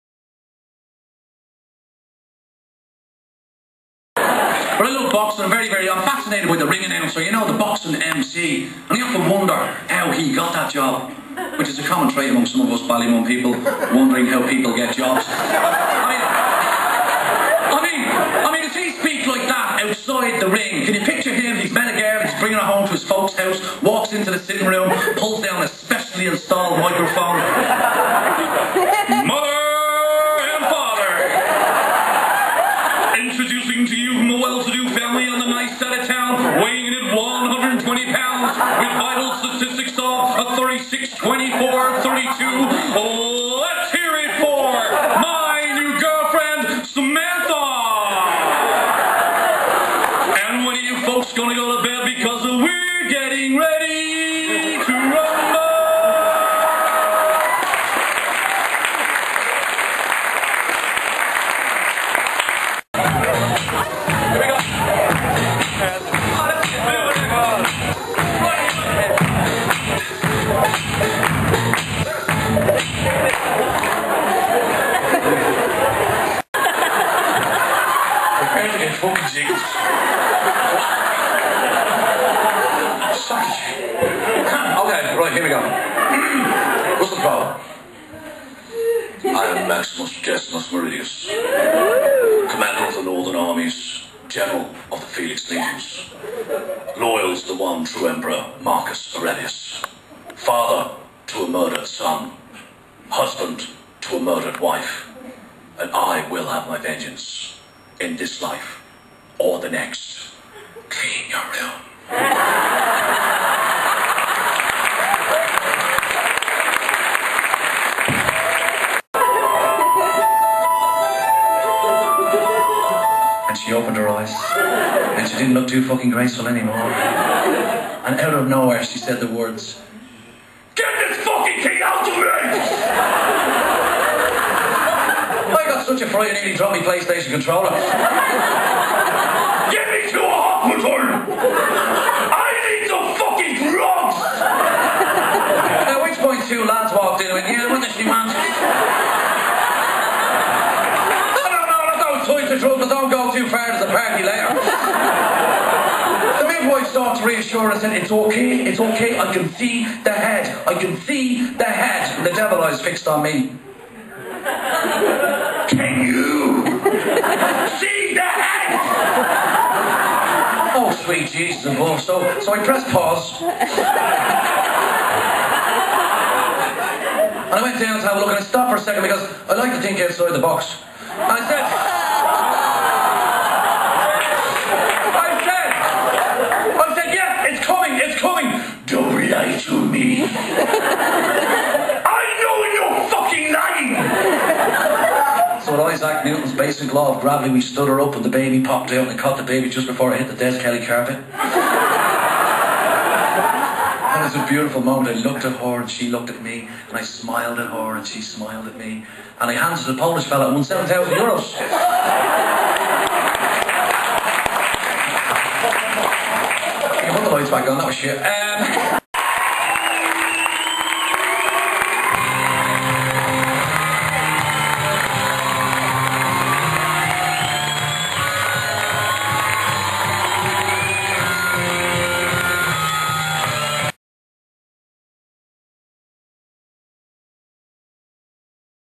Comedian Eric Lalor....BoB Dixon photography
The legend that is Mr Lalor playing to a packed out Axis in Ballymun